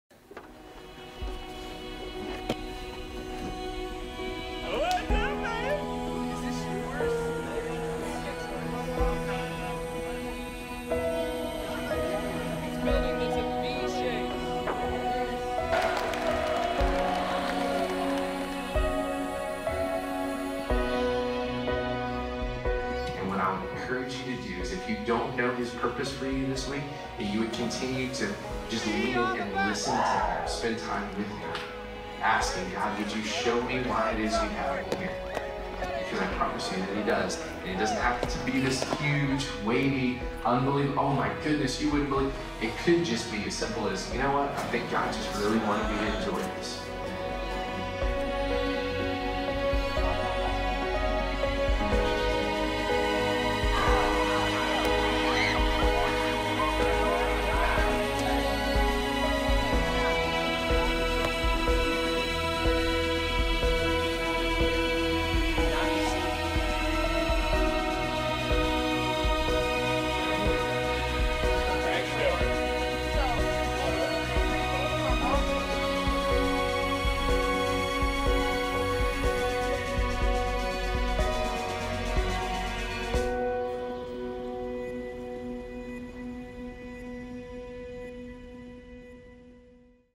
Generate Camp 2022
CovStudents! We are so excited to be attending Generate Camps this summer from July 11th-15th! Head to our website to get signed up today. Open to all 6th - 12th graders.